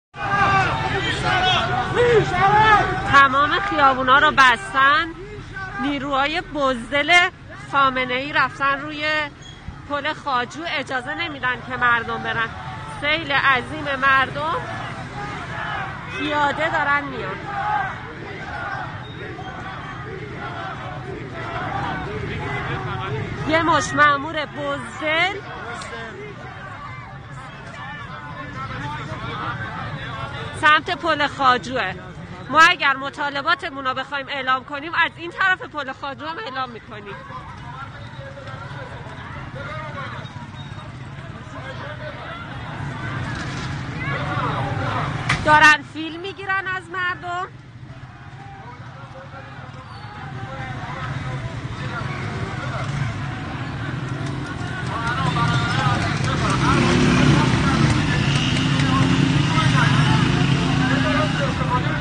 گزارش یکی از زنان معترض درتجمع گسترده اعتراضی اصفهان
نیروهای بزدل خامنه ای رفتن روی پل خواجو و تمام خیابانها رو بستن اجازه نمیدن مردم برن اما ما از این طرف پل خواجو هم خواسته هامون رو اعلام می کنیم. آنها دارند از مردم فیلم می گیرند.
جمعه ۵ آذر ۱۴۰۰